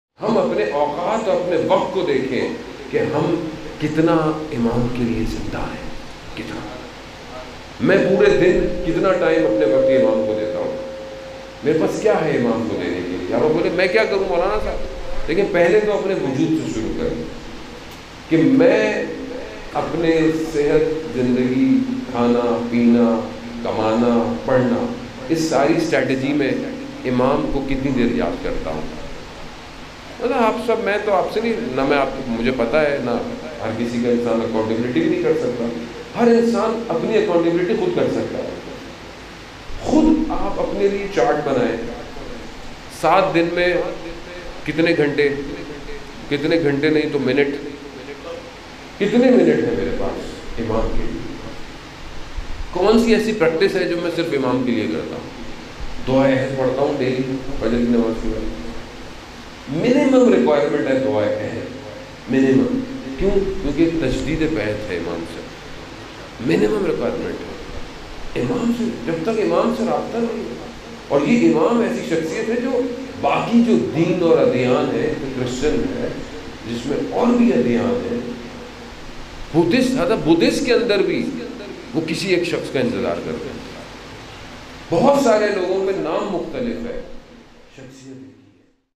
Meri Zindagi Me Imam kaha hai | Maulana Nusrat Abbas bukhari
Topic:- Meri Zindagi Me Imam Kaha hai

Speaker: Maulana Nusrat Abbas bukhari

courtesy:Al Qayam Collection

we upload short clip  only.